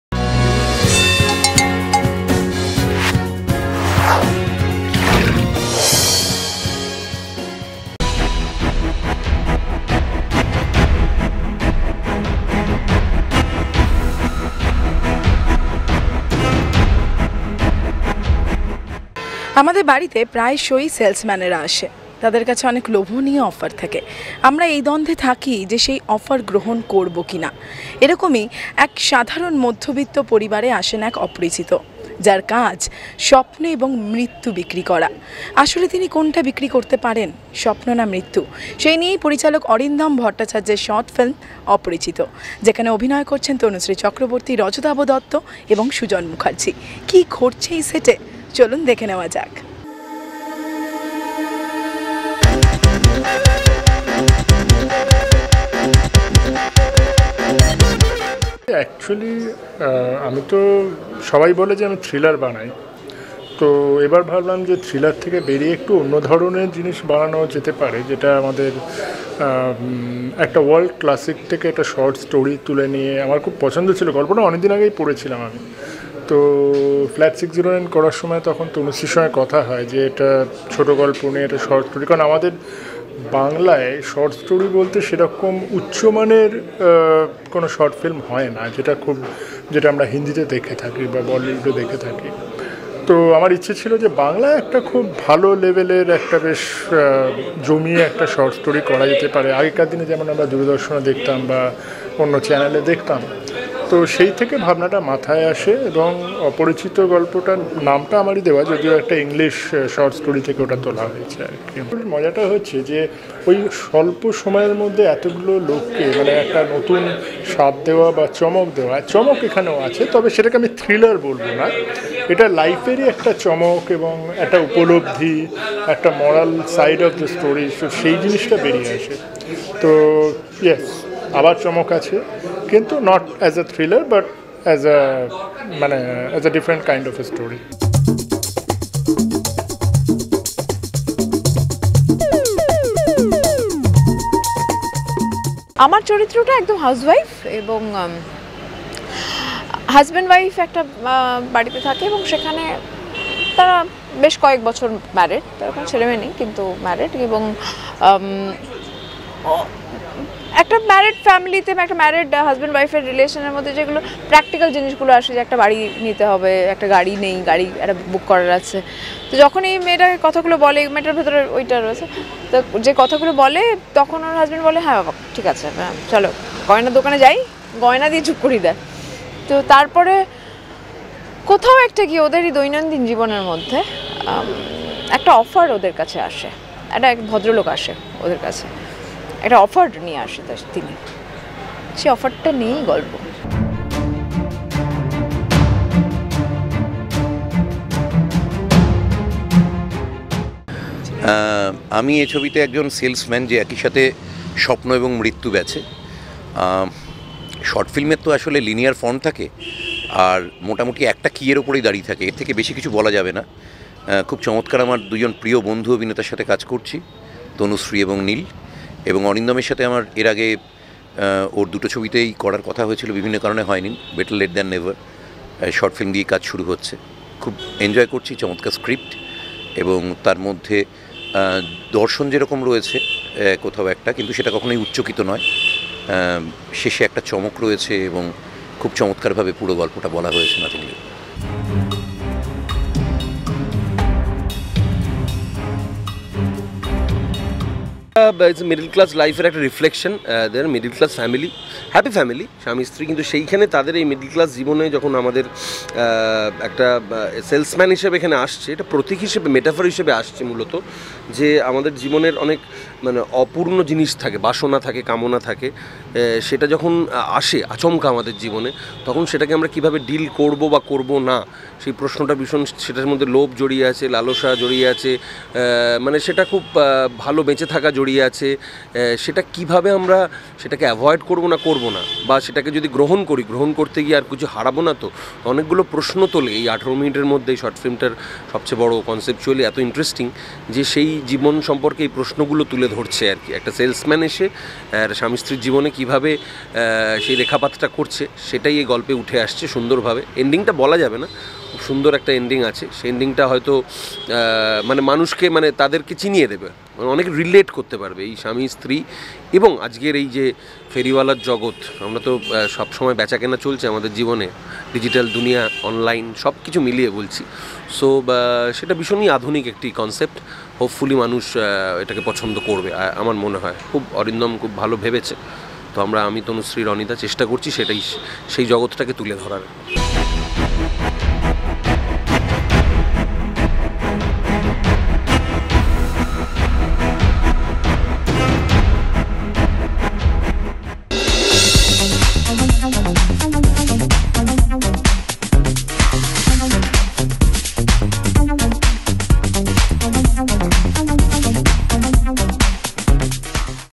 Oporichito | Short film Shooting | Tanusree | Rajatava | Neel | অপরিচিত এর শুটিং
Shooting coverage of short film Oporichito directed by Arindam Bhattacharya featuring Tanusree Chakraborty Rajatava Dutta and Neel Sujan Mukherjee produced by Raajdip